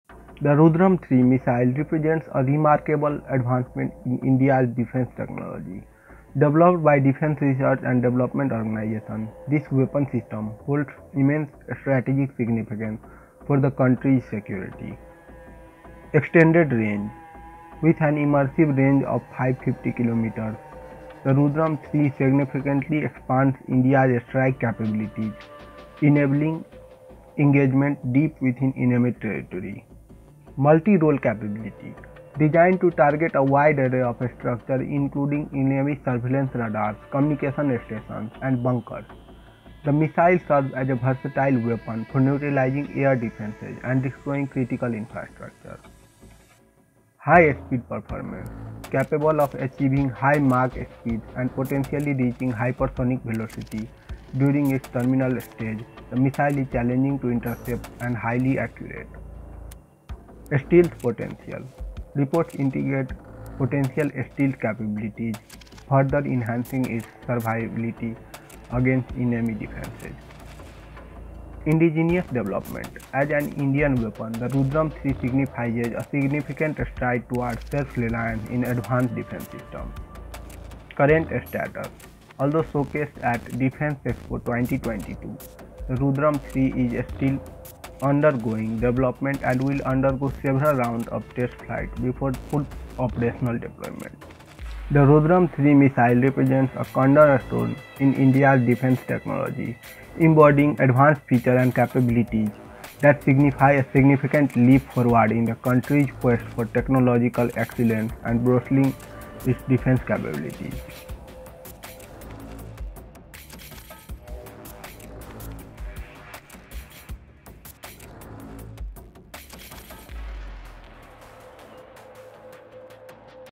Rudram-III Missile: Revolutionizing India's Defense Capabilities | Defense Affairs
The Rudram-III missile, developed by India's DRDO, revolutionizes defense capabilities with its 550 km range, multi-role design targeting varied assets, high-speed capability, potential stealth features, and indigenous development. It aims to bolster India's offensive reach, marking a significant leap in defense technology and sovereignty. Planned integration with fighter jets signifies its strategic importance in enhancing India's defense posture.

Explore the history, science, and technology of military engineering.
Chapters
0:00 Intro
0:14 Key Features
1:35 More Details